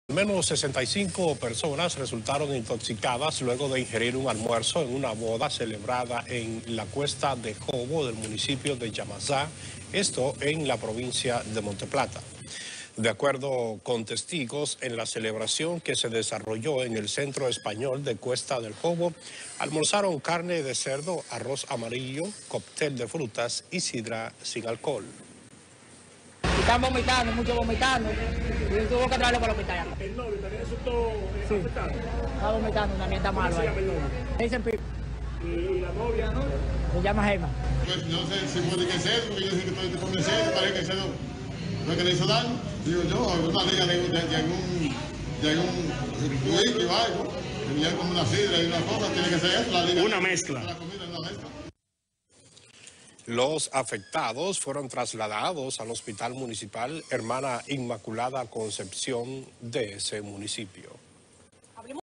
69 PERSONAS INTOXICADAS POR ALMUERZO DE UNA BODA
p. PAOLA VELASQUEZ